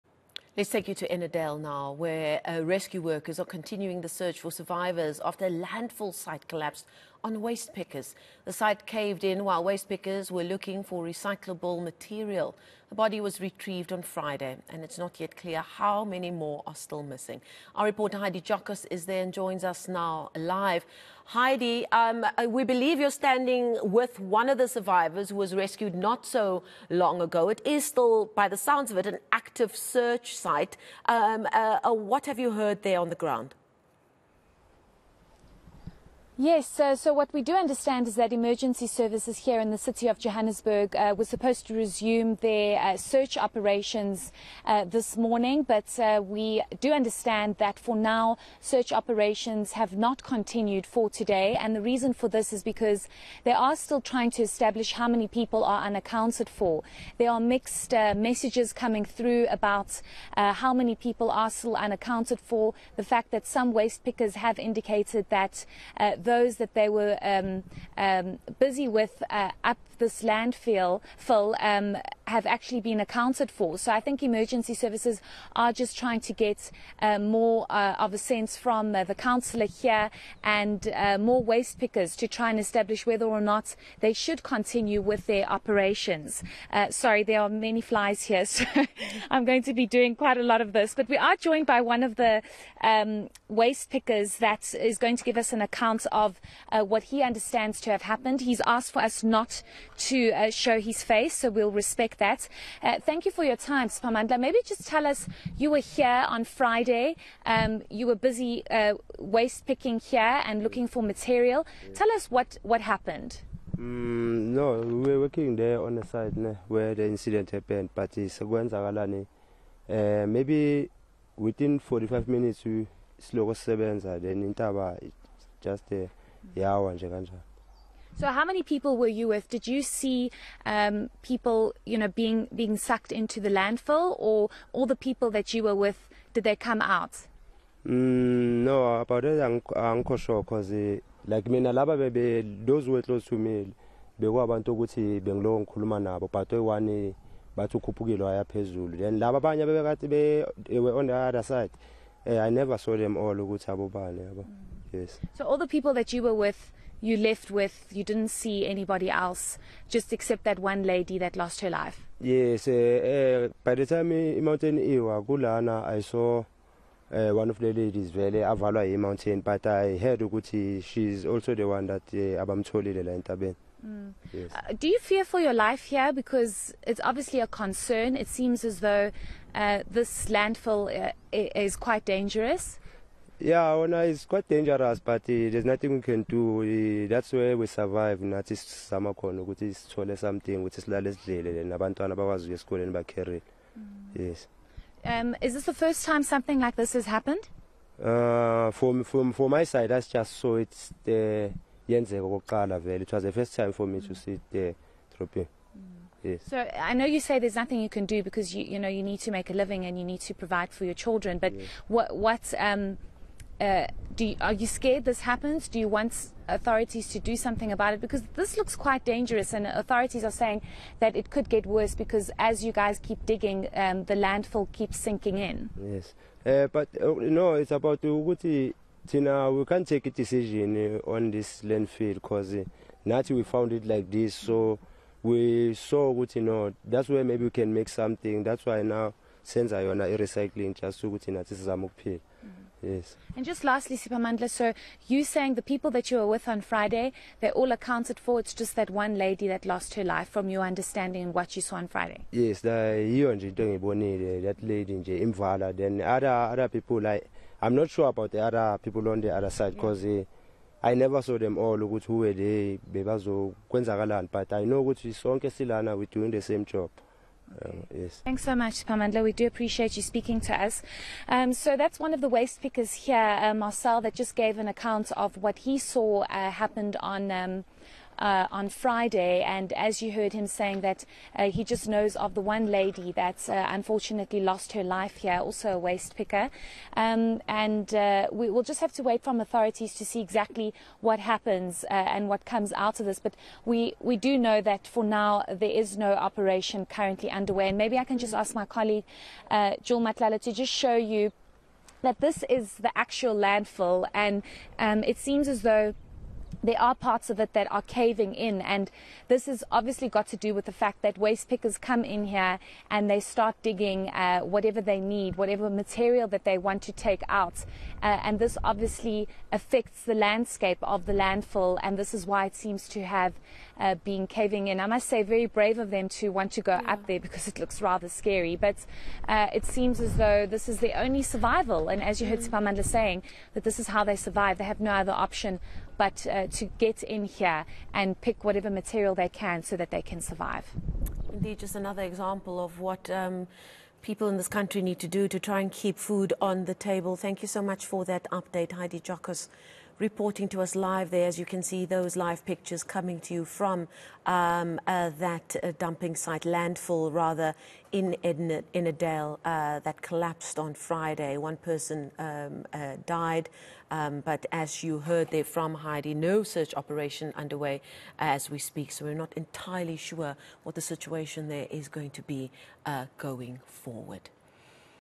Landfill Collapse | Search for missing waste pickers
Rescue workers are continuing the search for survivors after a landfill site collapsed on waste pickers. The site caved in while waste pickers were looking for recyclable material. eNCA reporter Heidi Giokos has more details.